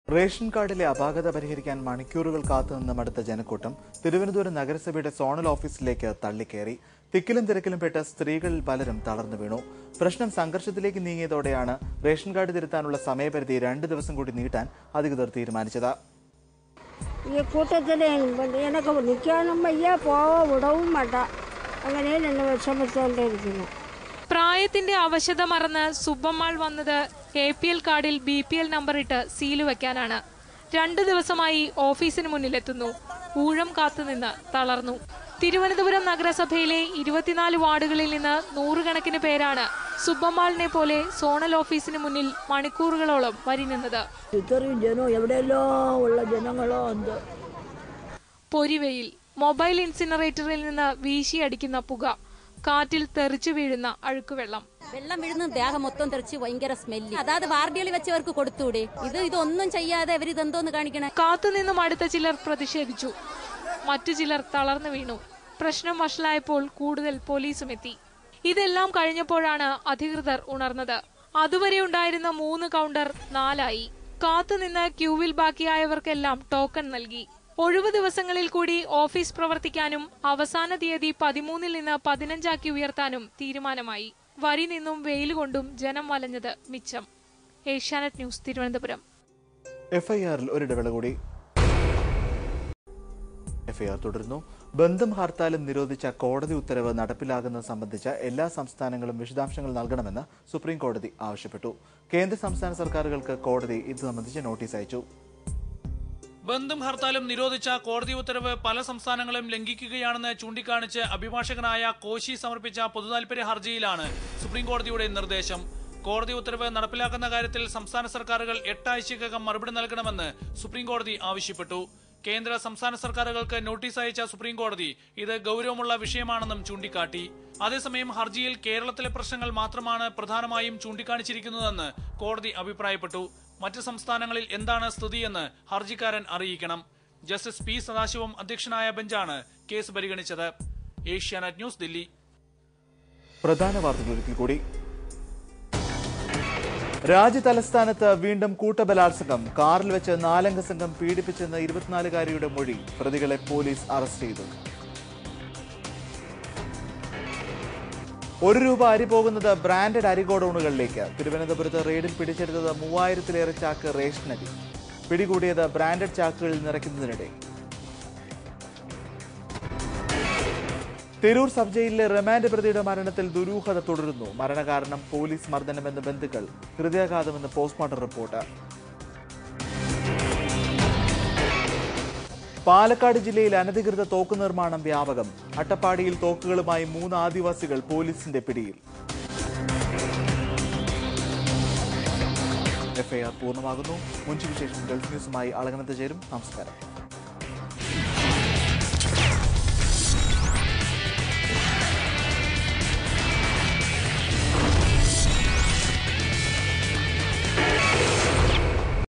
FIR 8,February 2013 Part 2
Stories:
1) Gang rape in car in Delhi
2) One rupee rice mafia in Kerala
3) Murder of remanded culprit in sub jail
4) Illegal gun makers in Attapadi